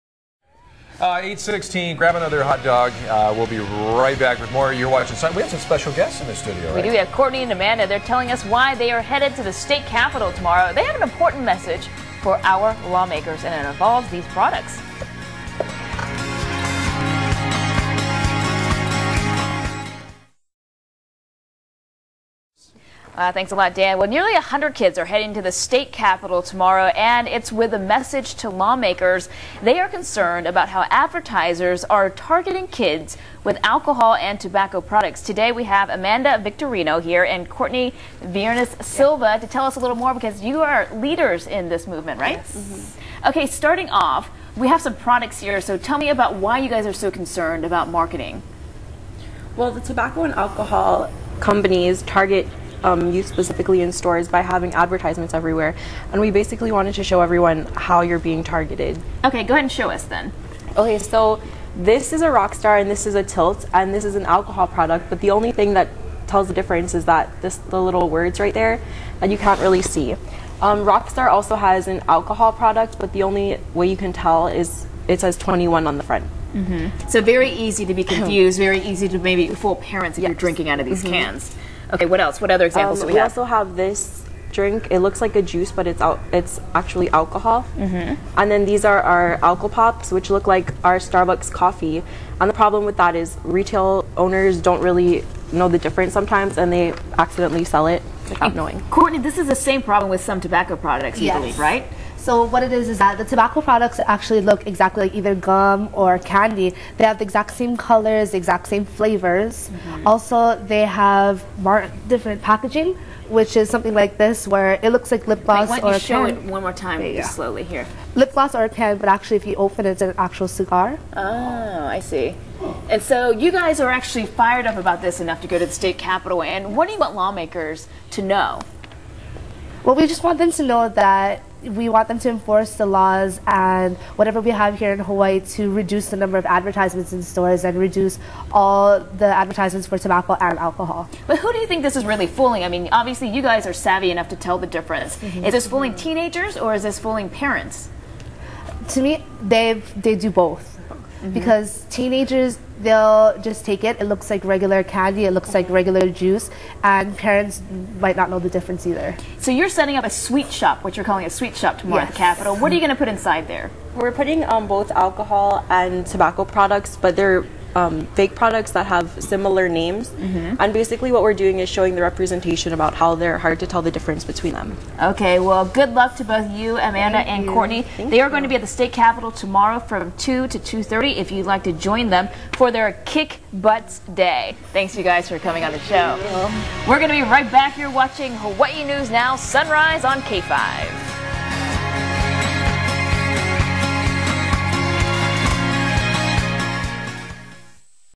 Hawaii Teens Stand Together Against Alcohol & Tobacco Advertising - Kick Butts Day 2010
Hawaii News Now - Nearly 100 kids will gather at the state Capitol Wednesday afternoon to take a stand against tobacco and alcohol advertisers. Courtney Viernes-Silva, 17, and Amanda Victorino, 17, tell us why.

Kick Butts Day

    * State Capitol rotunda
    * Tomorrow at 2pm
    * Survey results, storefront